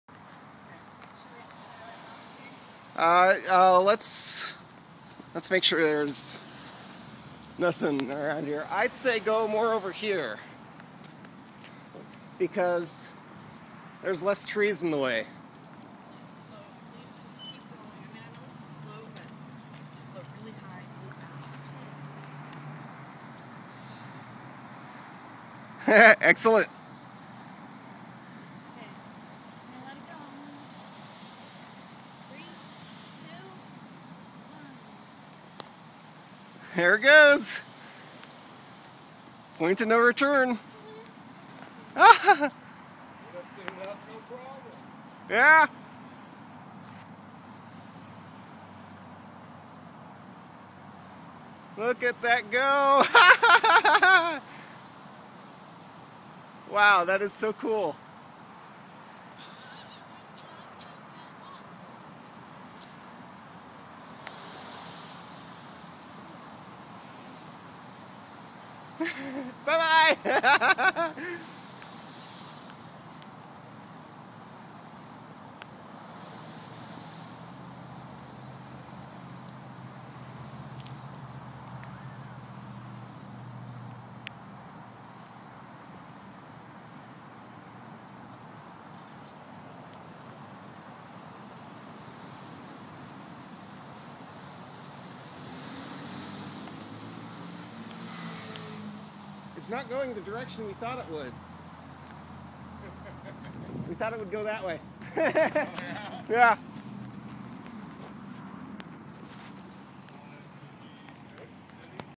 Balloon Launch SUCCESS
I assisted a high school student named Megan with a project consisting of the launch of a laptop into the upper atmosphere to take pictures and transmit them to the ground.  This was our second launch attempt, with the right amount of helium!  Forgive me, I get a little giddy in this one.